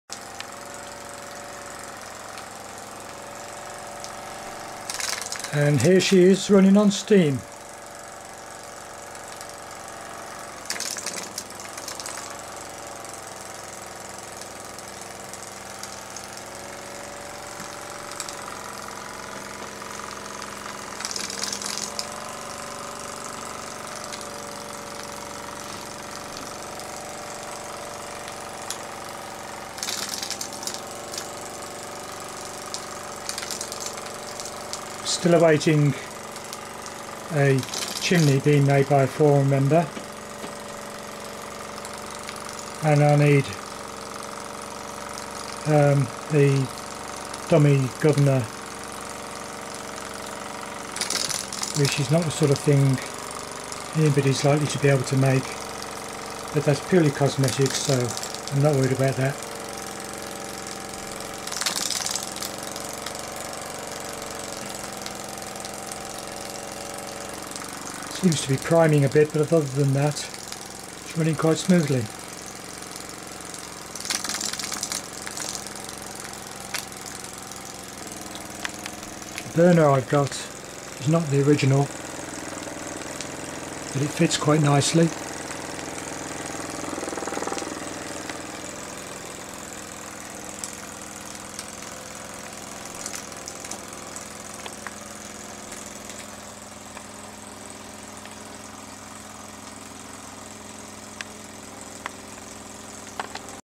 EP Hercules steam test
Ernst Plank "Hercules" steam engine - first steam test after repairs.